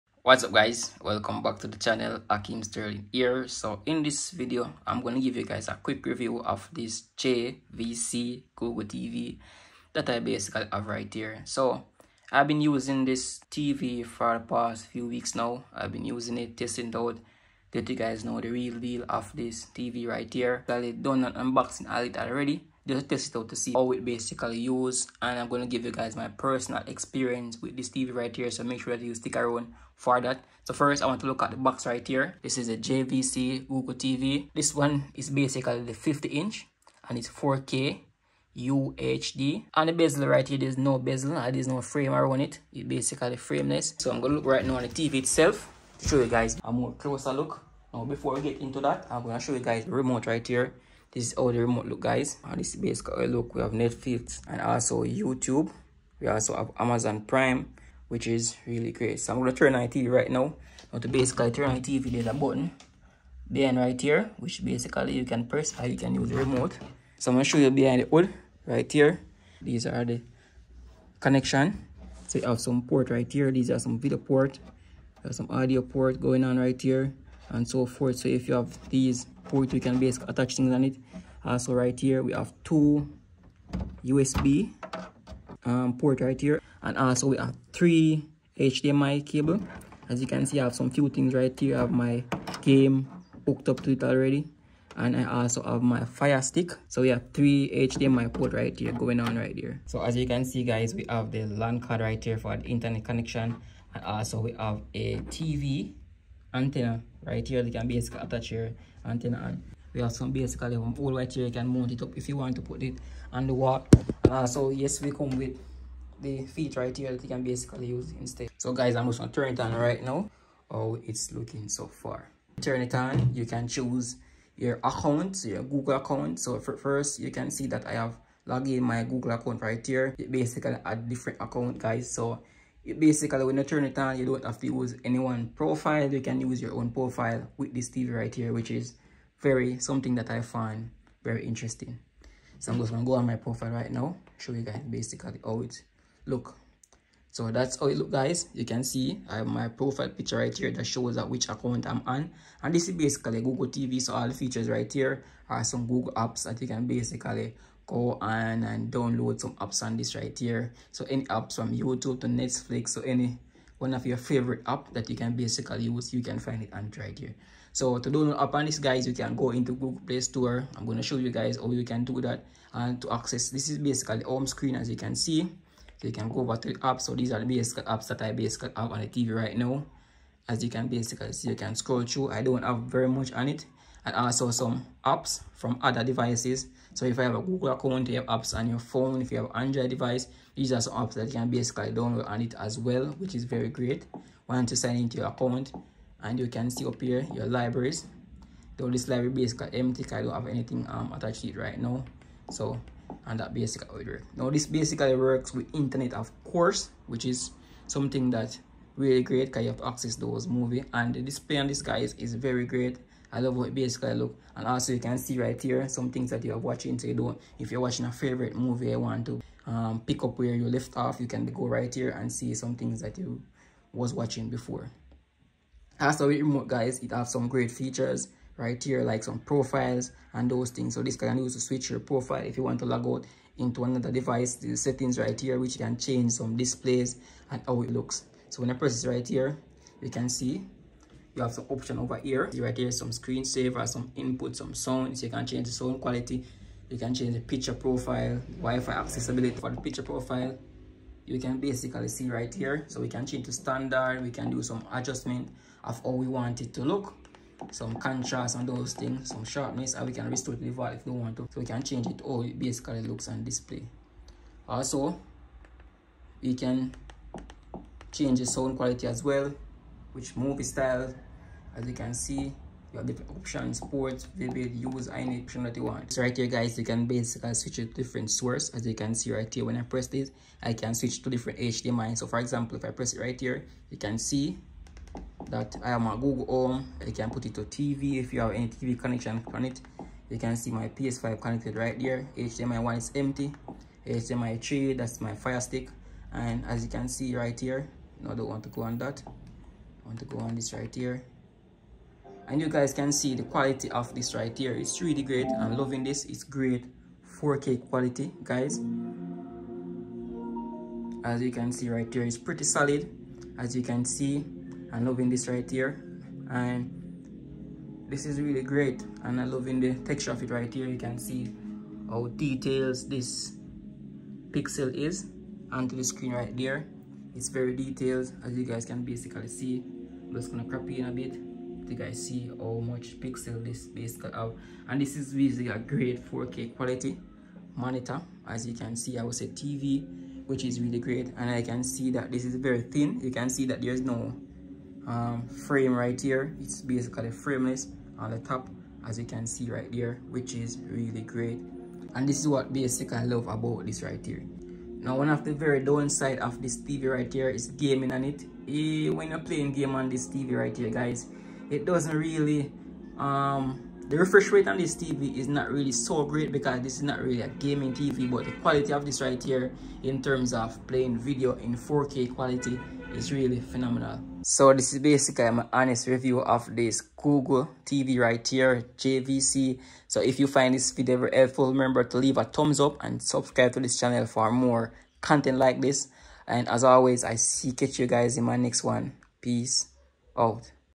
My Review: JVC 50-Inch 4K (UHD) Smart Frameless TV Compatible with Netflix, Amazon Video and YouTube
JVC 50-Inch Class 4K (UHD) LED Smart TV Compatible with Netflix, Amazon Video and YouTube

My Equipment
Phone Camera
DJI Mini 3 Pro
Fly More Combo
Movo Mic

Other Accessories

Memory card i use
Power bank i use
Hard drive